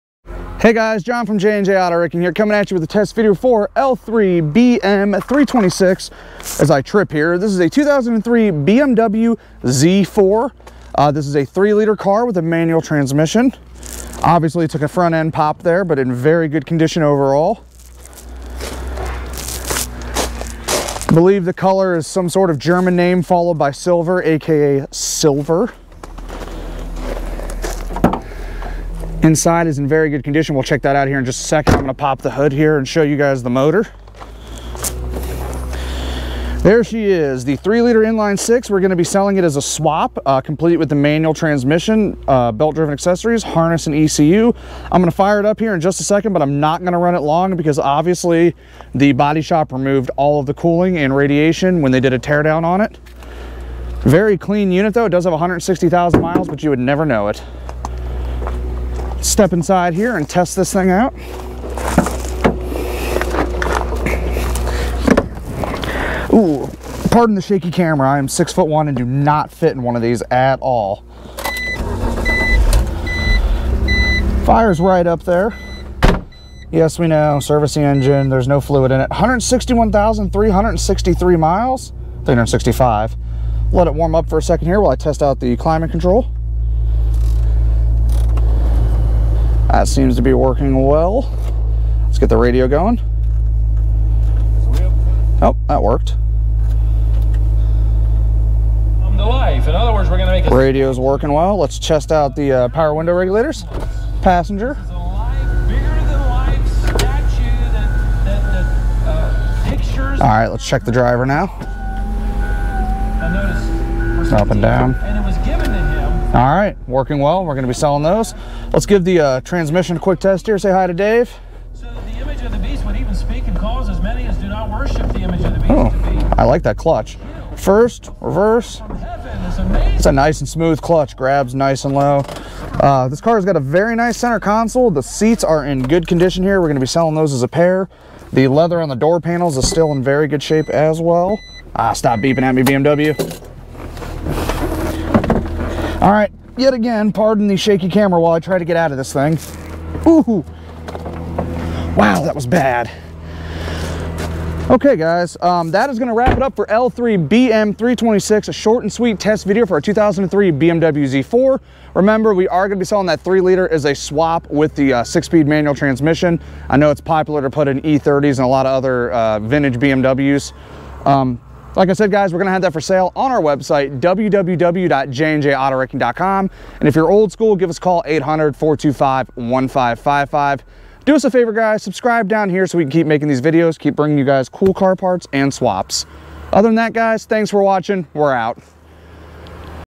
2003 BMW Z4 3.0L M54/ 6 Speed Manual Swap Donor Car l3bm326
In todays episode we check out our 2003 BMW Z4 M54 Swap Donor Vehicle. The entire drivetrain is being sold complete as a swap for your custom project.